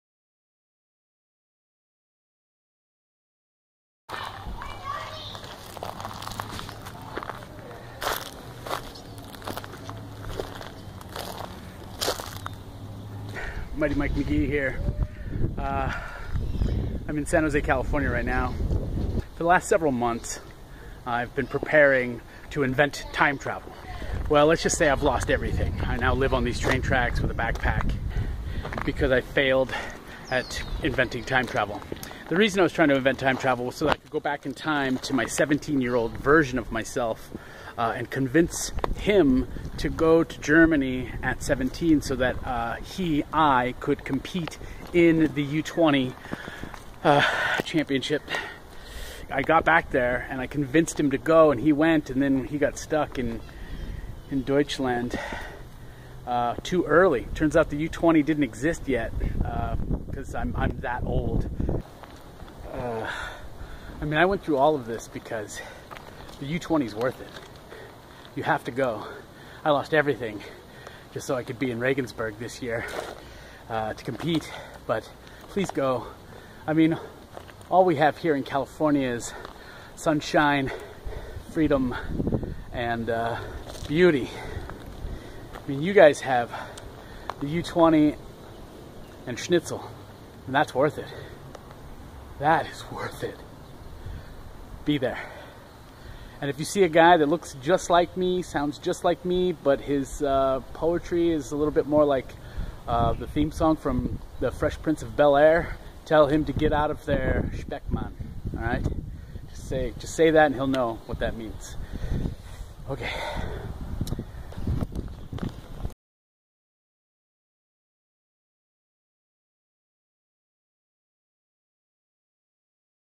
Mighty Mike McGee For U20 Slam 2015
Thanks to bad time travel planning, McGee is not coming to Regensburg, Germany for the U20 Slam 2015. He is still too old to compete in the youth slam next month. This is unfortunate. He lost everything and now he cannot attend.

But you should!